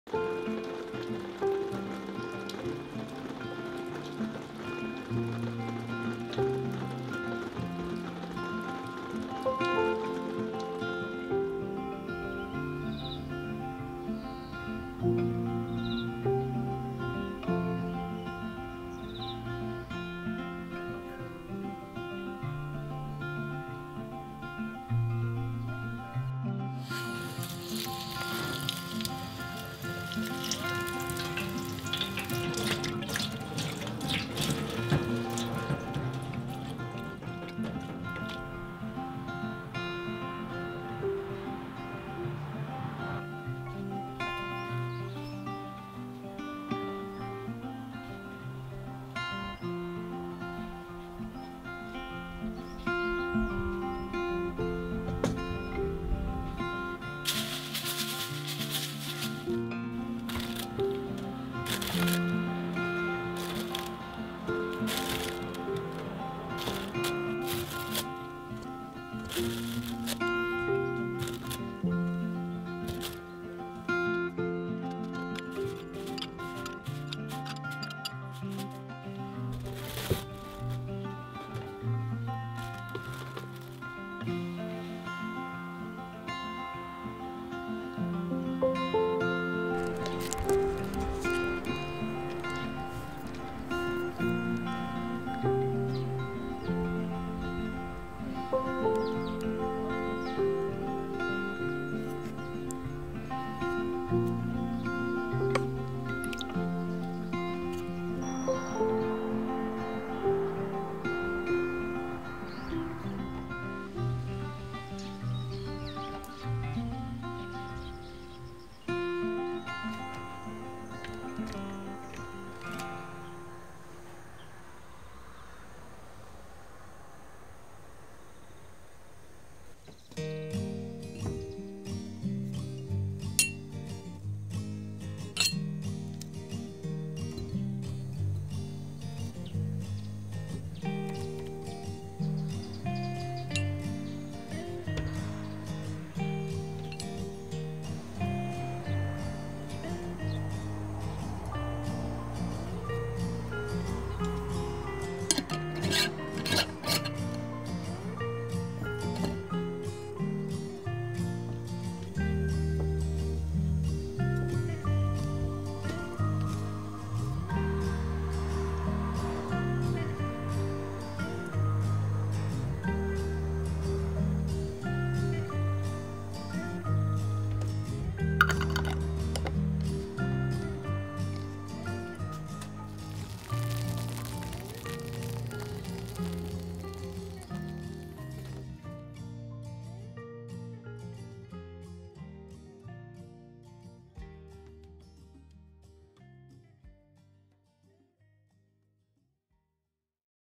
友人がくれた無農薬の南高梅で梅干しを作る「手間を楽しむ田舎暮らし」
東京から田舎に移住して、5年目を迎えた。
友人に無農薬の南高梅をいただいたので、梅干しを作る。
不揃いで、傷もあるが完熟した梅から甘い香りが溢れ出る。
今回は、持ち運び用に梅酢なし、食卓でいただく用に梅酢ありの
2種類の梅干しを作る。

三重県御浜町ついて詳しくは下記Webサイトをご覧下さい↓